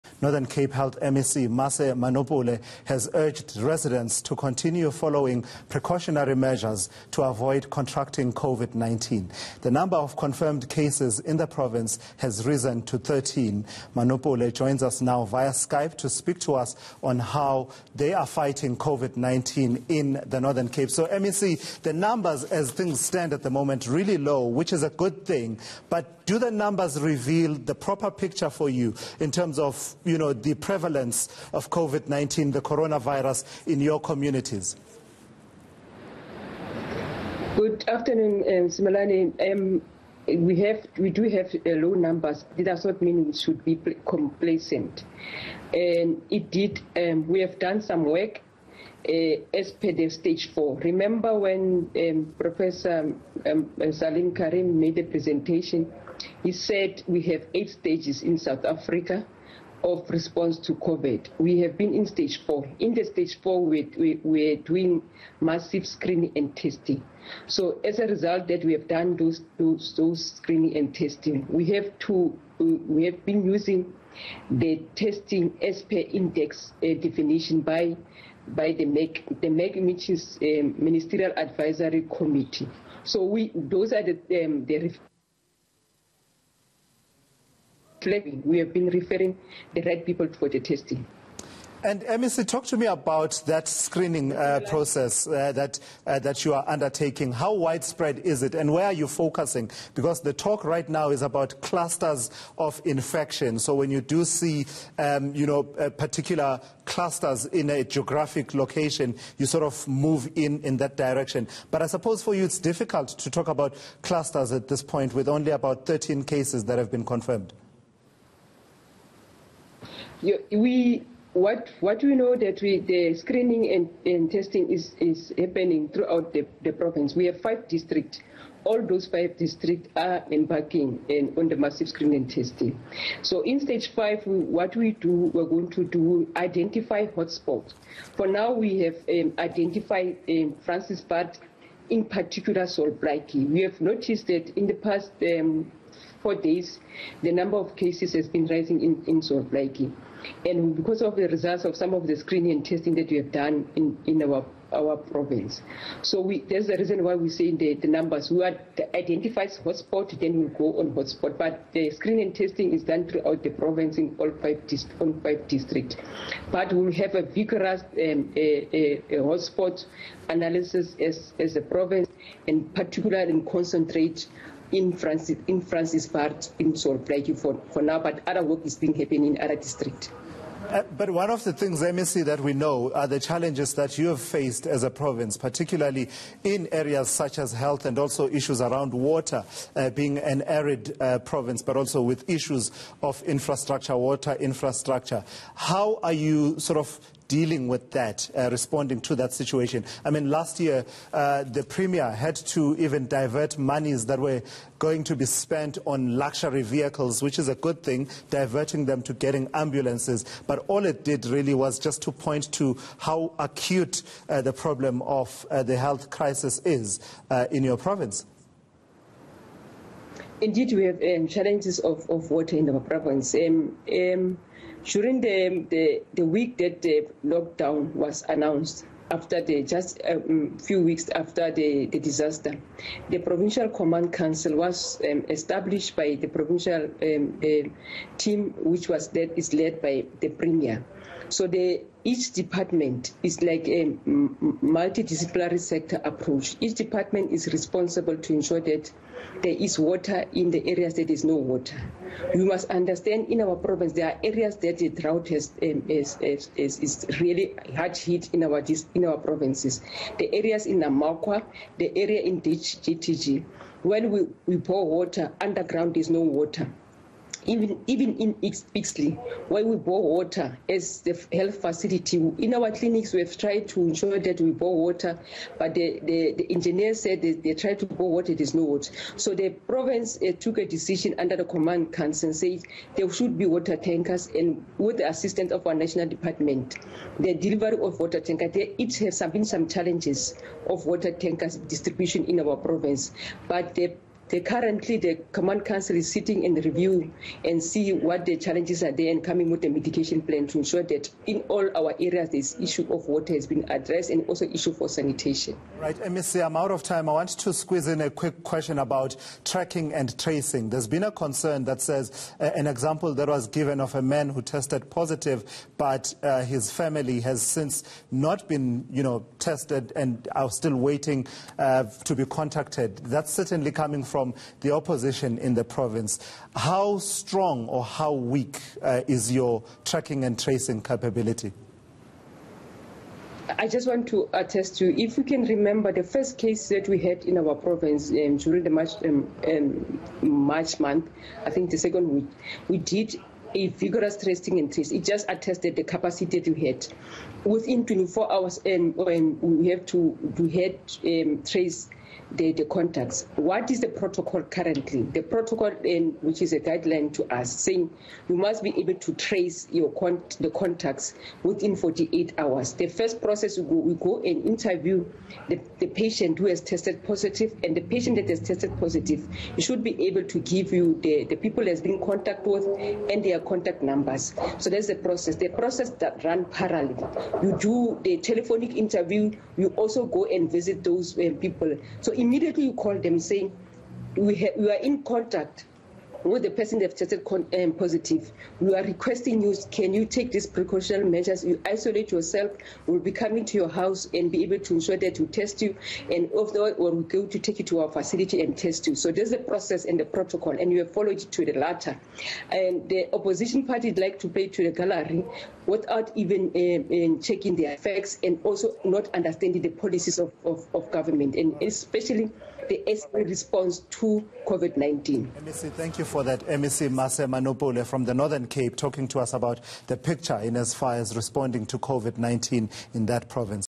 Northern Cape urges prevention
Northern Cape Health MEC Mase Manopole has urged residents to continue following precautionary measures to avoid contracting COVID-19. The number of confirmed cases in the province has risen to 13. day41oflockdown Courtesy of DStv403